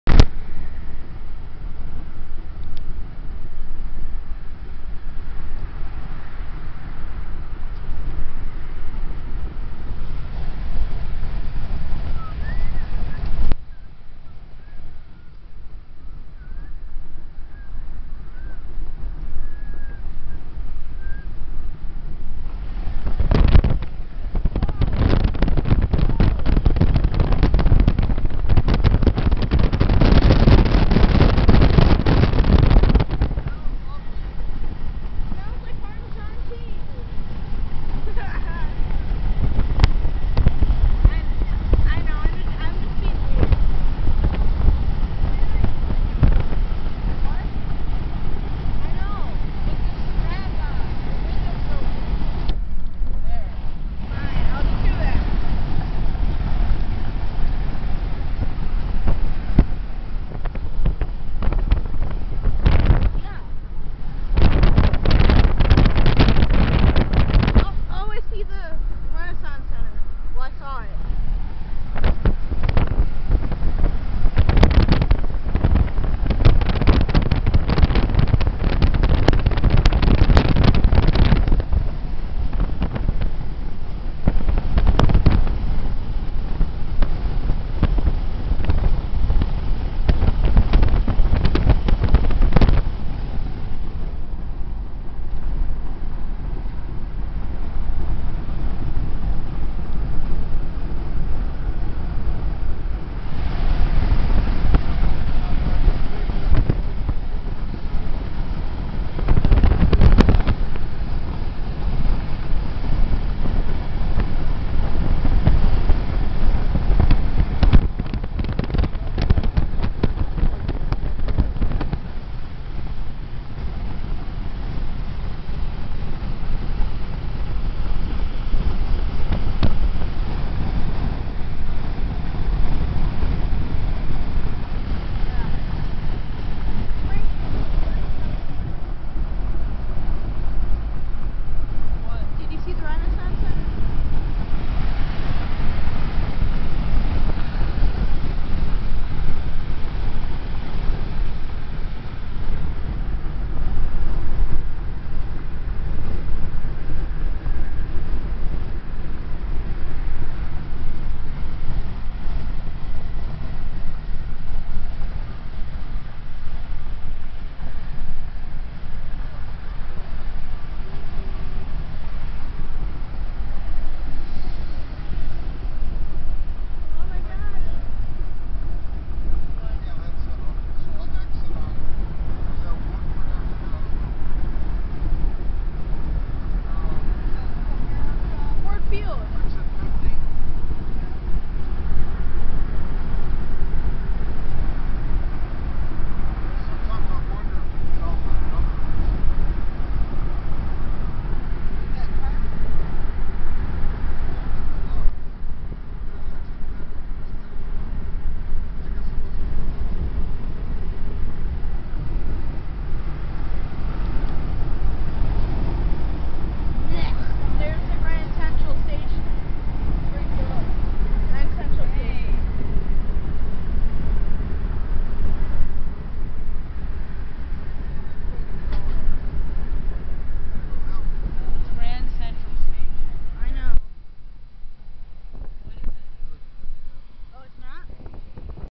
Trip to Detroit
We were going to the Paul McCartney concert in Detroit. So, I filmed some of the buildings.